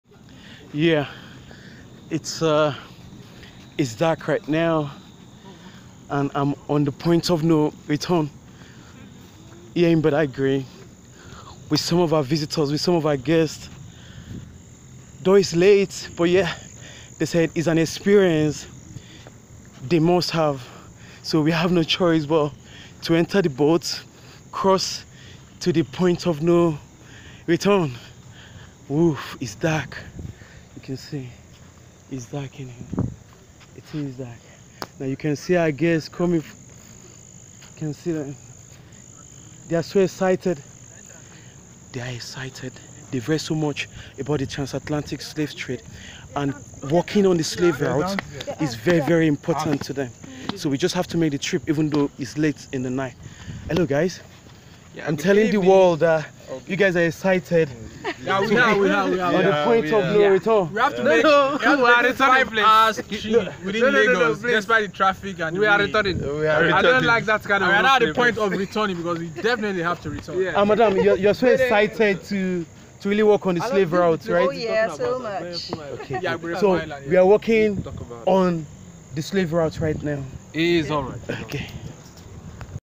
Badagry Night Tour Excitement at The Slave Point of No return
The excitement on this trip is the eagerness to learn about the past as the voices of the past continue to speak to the present.

This creative night tour on the slave point of no return evolved as a coincidence. The lessons to learn quickly unfolded from the beginning. The people really want to learn their history and have a practical experience irrespective of the consequence.

The excitement here is the anxiety to learn about the past. This is the first time a tour would be held at night on the Slave Point of no return, Gberefu, Badagry and organized by Anago Osho Adventures.

The touring group were supposed to come for a day-time tour but were deterred by the Heavy Lagos rainfall that held them in traffic for over five hours.

The slave route experience on the point of no return was important to them.

The touring group saw a slave history documentary sometime ago and had a group discussion and decided to have a practical experience after reading about Anago James Akeem Osho, who is an historian, and proclaimed reincarnated former Slave, Slave history and Cultural Tour Guide.

The touring group eventually arrived Badagry around 6.58p.m and were received by Anago Osho Adventures team.

Anago James Akeem Osho advised the group to stay in an affordable hotel for the night and continue the tour the following morning but the group insisted that they follow the itinerary that night.

The trip lasted for about three hours and the boat experience and the walk on the slave route was mind boggling.

Pictures were taken in the dark with the aim of seeing former enslaved people appear in the pictures.

The Lagos state government and the Federal government of Nigeria should light up the water ways and improve marine tourism infrastructure.

History is for us to learn from.